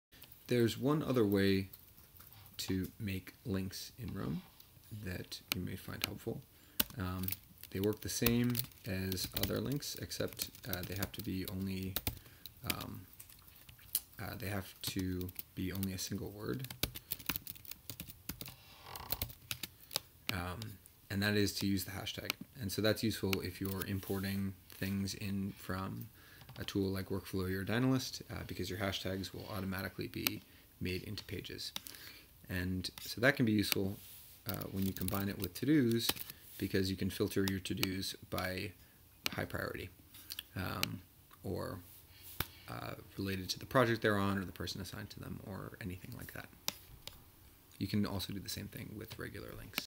Tags in Roam
Tags as an alternate form of links, and their use with TODOs